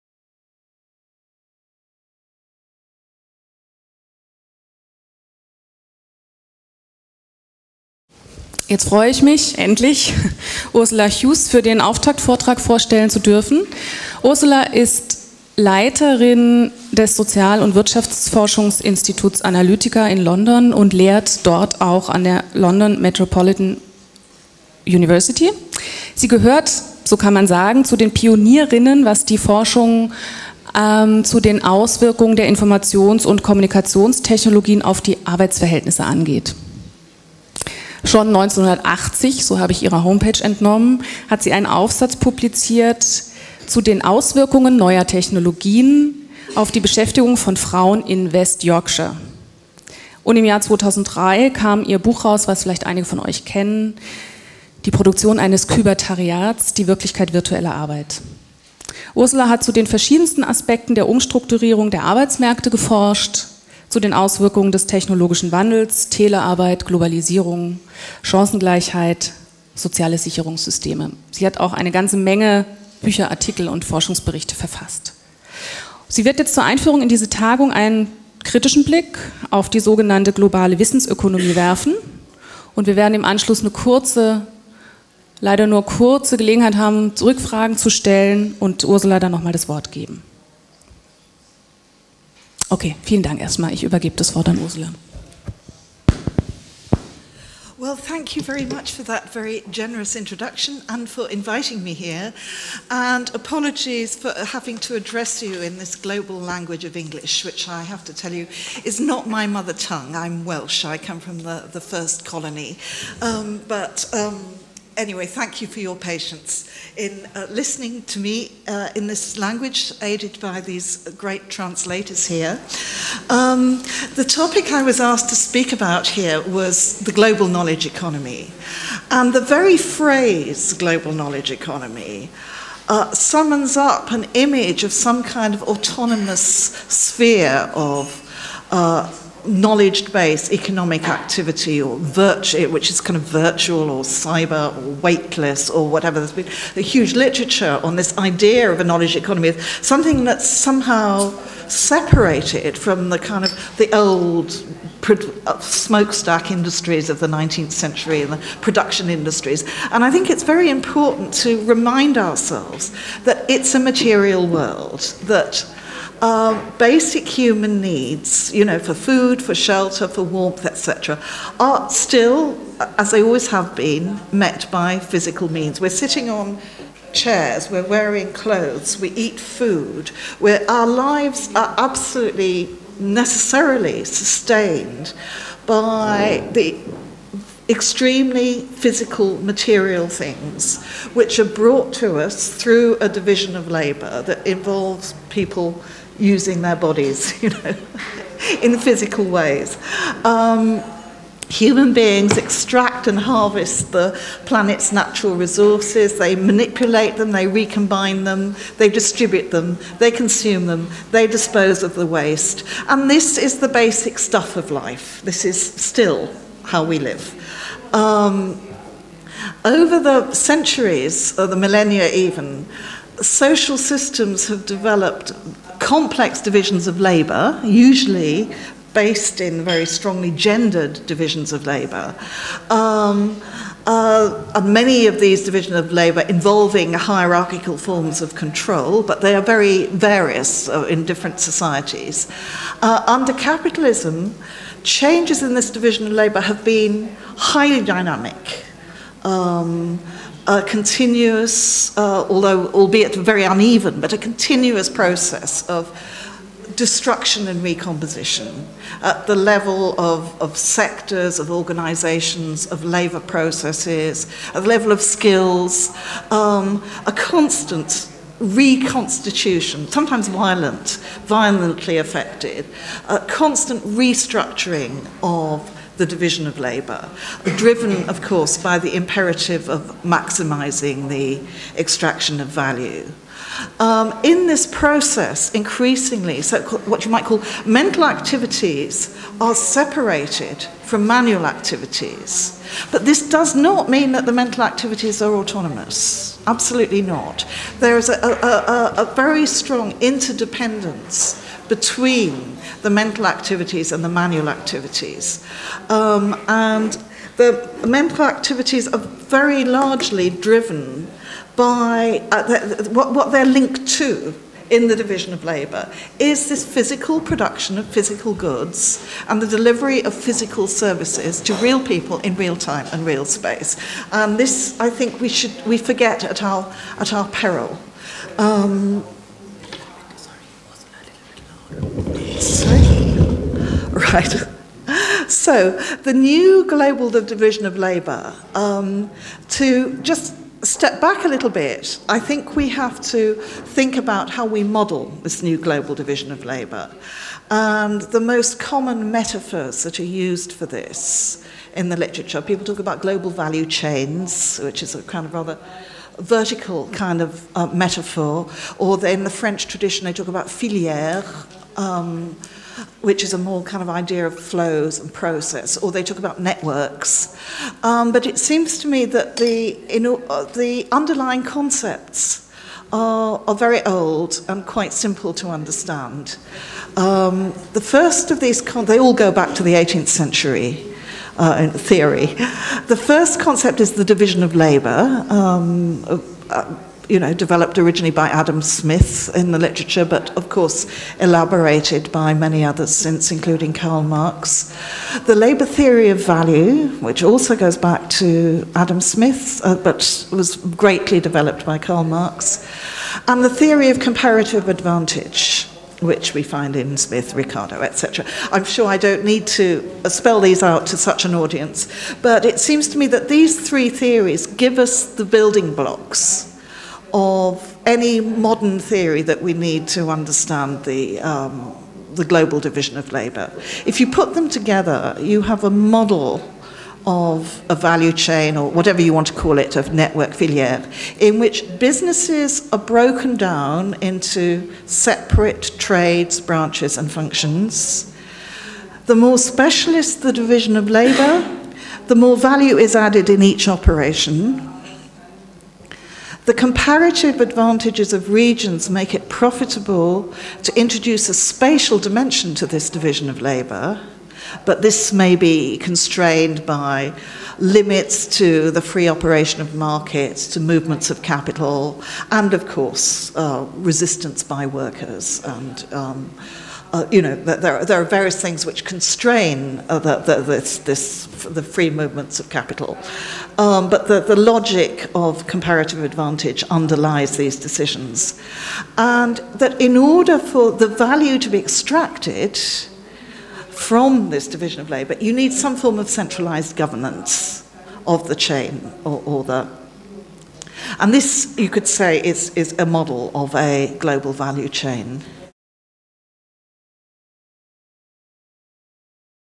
Huws: Die globale Wissensökonomie (Zusammenschnitt)
Ursula Huws, International Labour Studies, London: Key-Note zur Konferenz "Kapitalismus dot com" (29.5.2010, Berlin), Ausschnitt. Vollständige Aufzeichnung